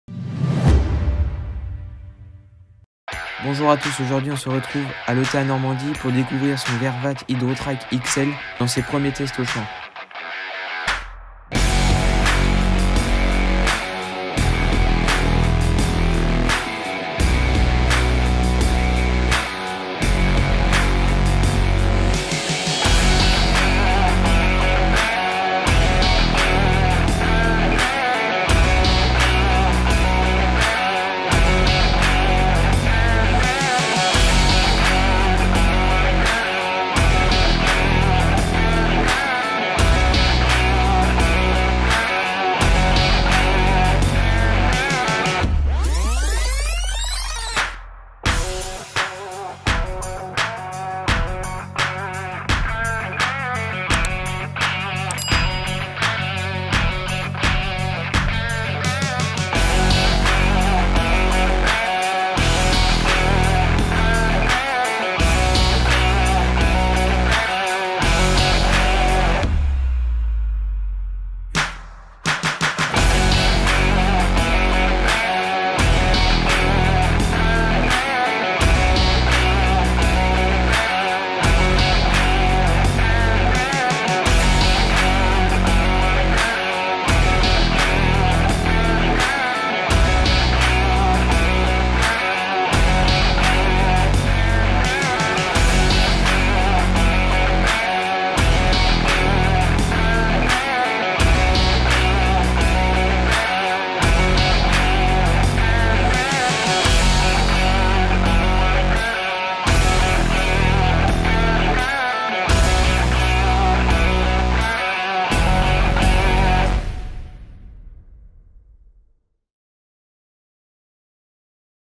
🤩Cet automoteur de 530 ch en action à l’ETA Normandie🇫🇷 VERVAET HYDRO TRIKE XL💨
En ce dimanche matin, on se retrouve pour une nouvelle vidéo à l’ETA NORMANDIE 🇫🇷

Dans cette vidéo nous allons découvrir le nouveau Verveart à l’épandage de lisier comportant près de 21 000 l de capacité de cuve😍 Cette automoteur doté d'un moteur de 530ch et de ces 5 roues qui convient a un travail en condition humide 😎
Merci à l’ETA NORMANDIE 👋